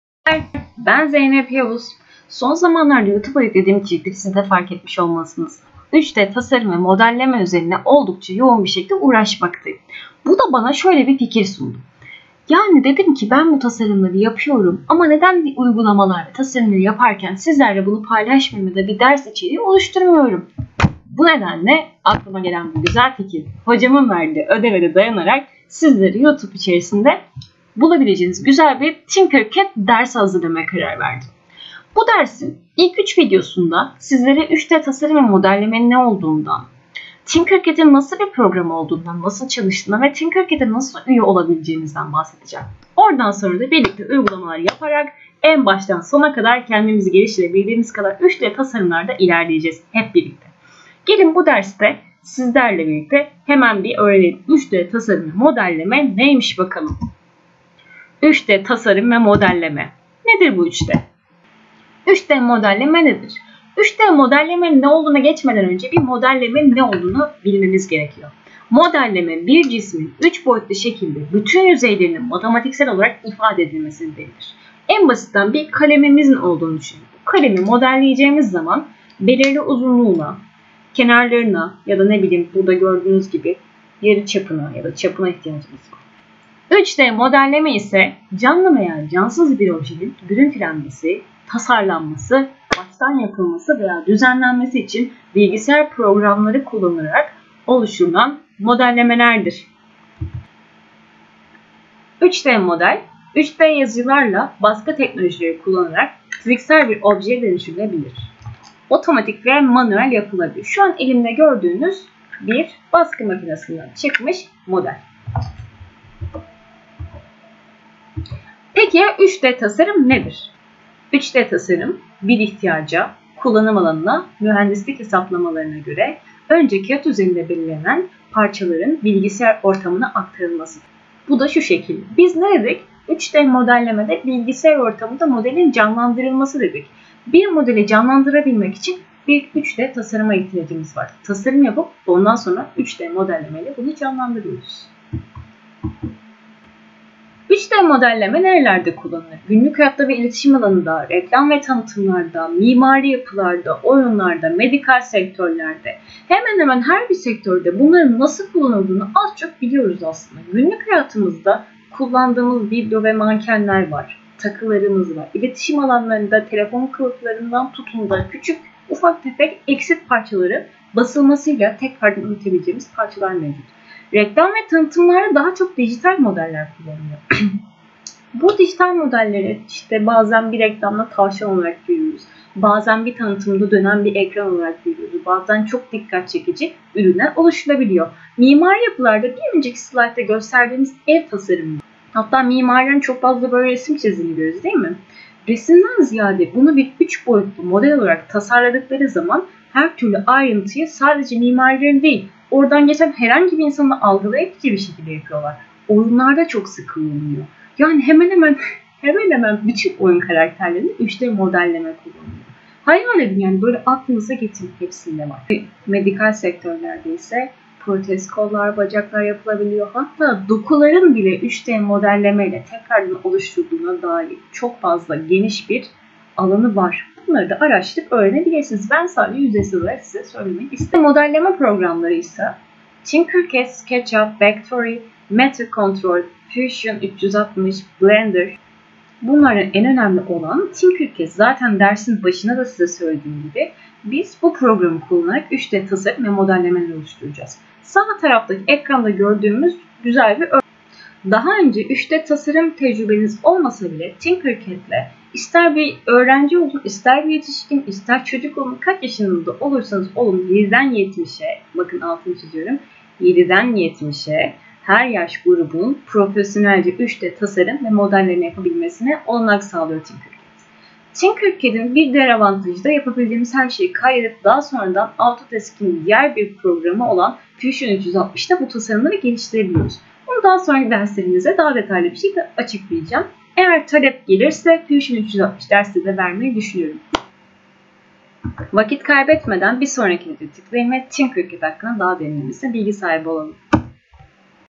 Tinkercad Dersleri 1- 3D Tasarım ve 3D Modelleme Nedir?
Youtube yeni içeriğindeki ilk video ile sizlerleyim. Hep birlikte baştan sona uygulamalarla Tinkercad öğrenelim.
İlk video içeriği 3d tasarım ve modelleme kavramları üzerinde duruldu. Bir sonraki videolarda Tinkercad nedir nasıl kayıt olup bir hesap oluşturulur bunun üzerinde duracağız. Takipte kalın.

Bu serinin sonunda Fusion 360 rehberi istiyorsanız lütfen yorumlarda belirtin.